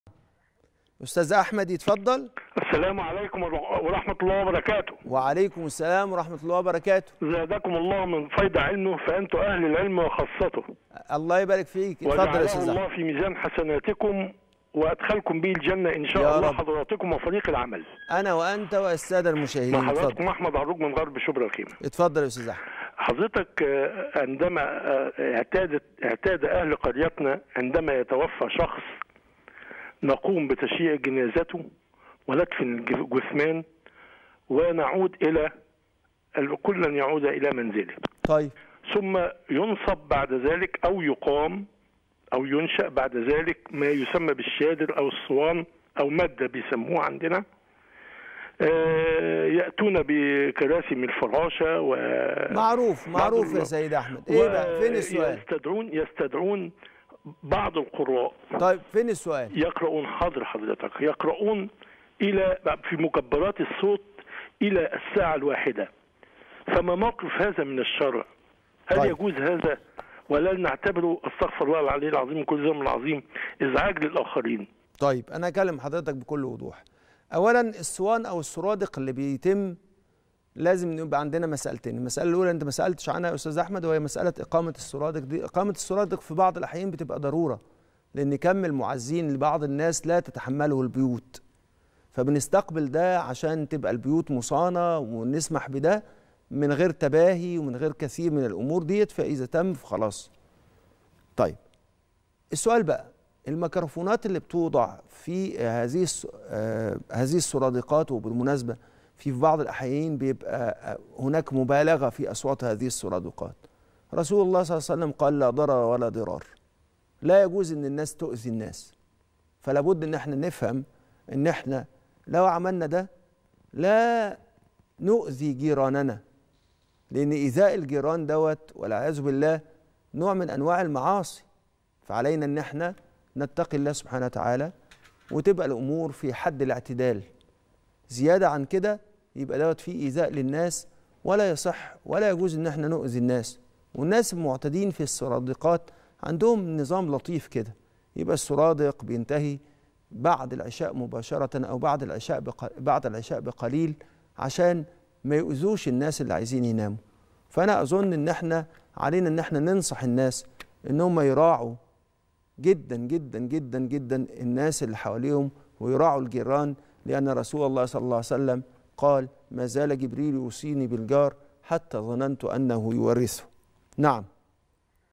ولا تعسروا | حكم إقامة العزاء وفتح مكبرات الصوت .. رد د. عمرو الورداني
حكم إقامة العزاء وفتح مكبرات الصوت .. رد د. عمرو الورداني